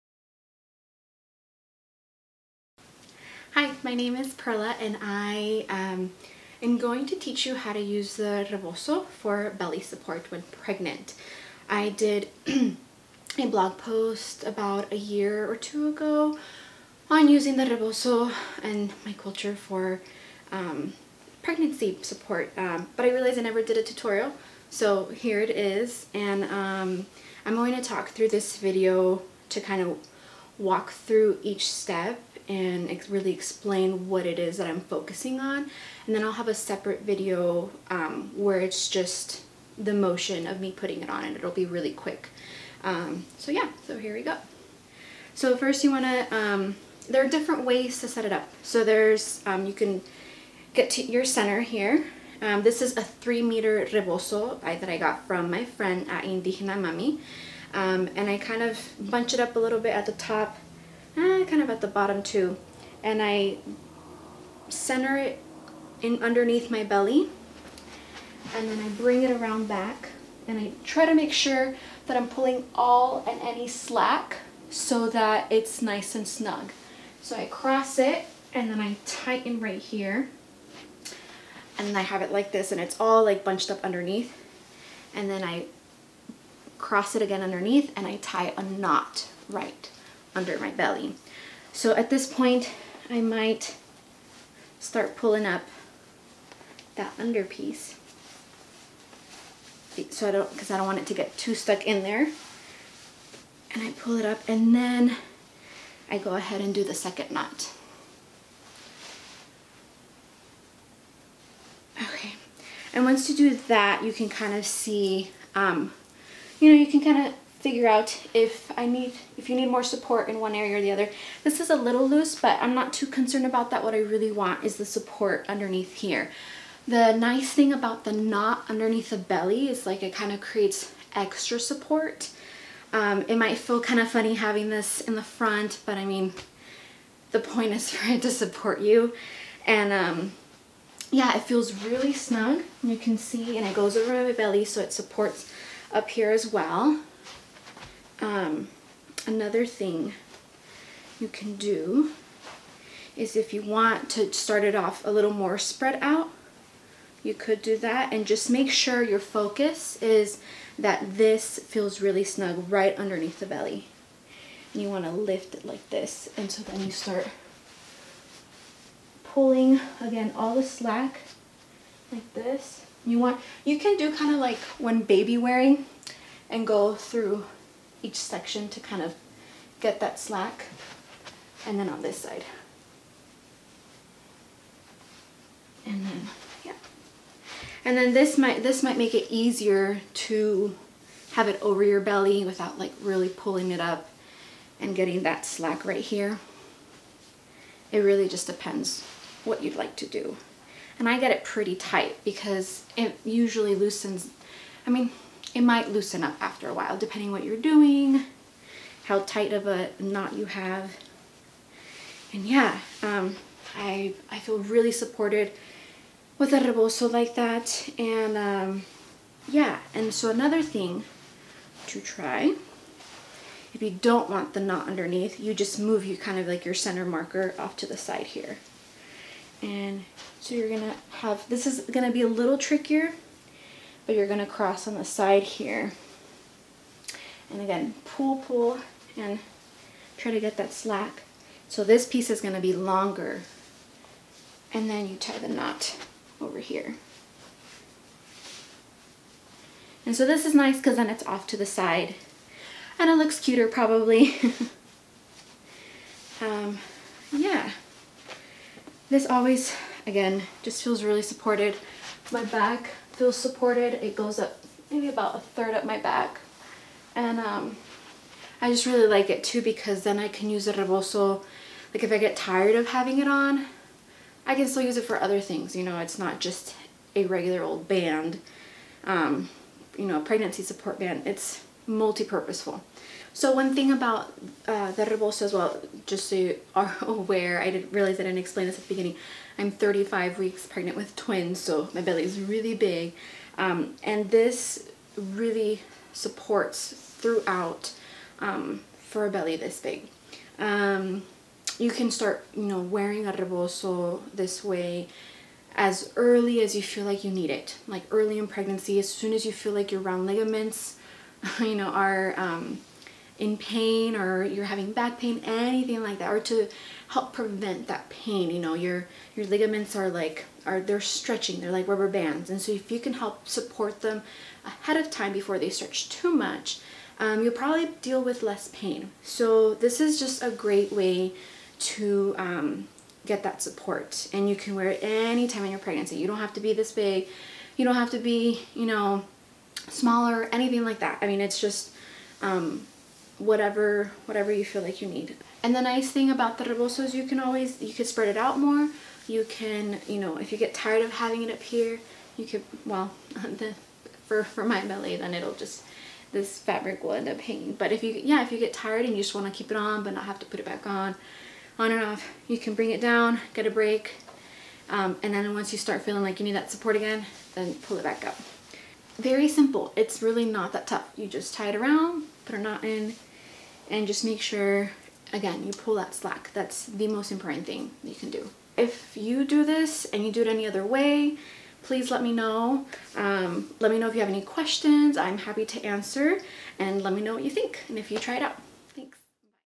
Rebozo Support for Pregnancy
Sharing how to use a rebozo for belly support when pregnant. Here I am 35 weeks pregnant with twins and have been using the rebozo since early in my second trimester. This is a 3 meter rebozo from Indigena Mami which you can find on their site.

You can learn more about using the rebozo during pregnancy on my blog as well.

It is not only support but sacred knowledge for us to reconnect to our ancestor's healing ways.